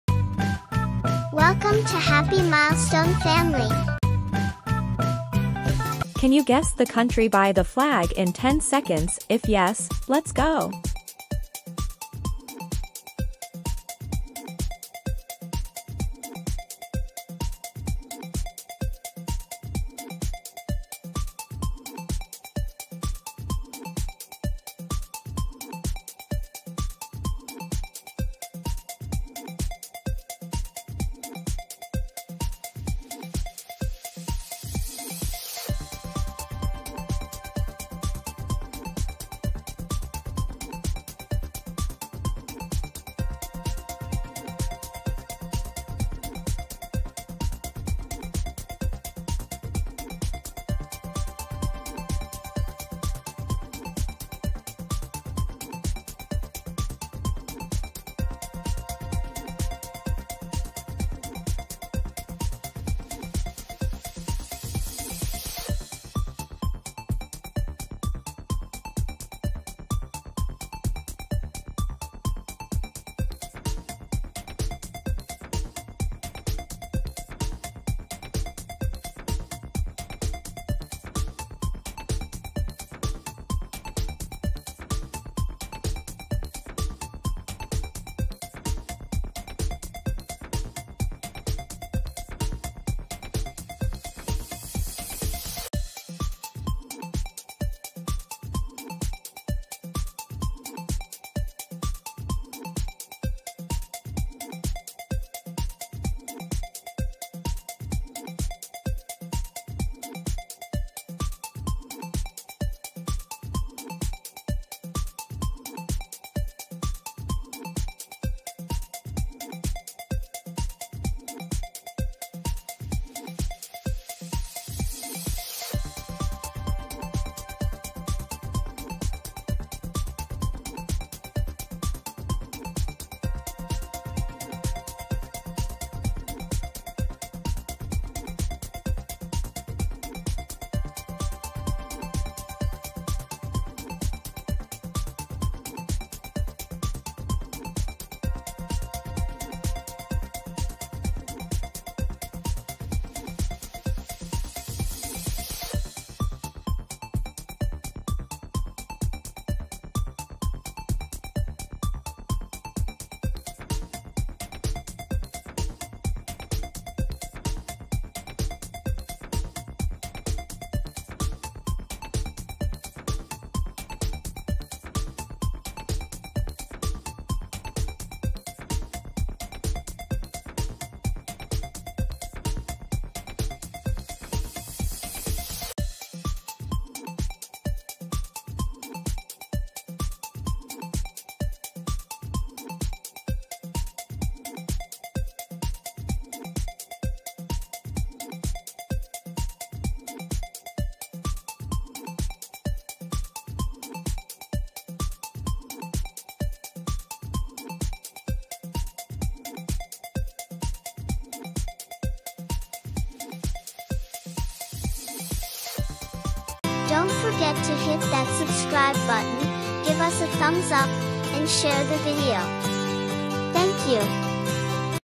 Guess the Country by the Flag Quiz 1 I Can you Guess the Flag I Fun Quiz
"Test your knowledge of world flags with our exciting 'Guess the Country by the Flag' quiz! Challenge yourself and friends to identify countries based on their national flags. From well-known flags to the more obscure, this fun and educational quiz is perfect for geography enthusiasts of all ages. Whether you're a seasoned traveler or just curious about the world, you'll enjoy discovering new facts and brushing up on your flag recognition skills. Play along, learn as you go, and see how many countries you can guess correctly!"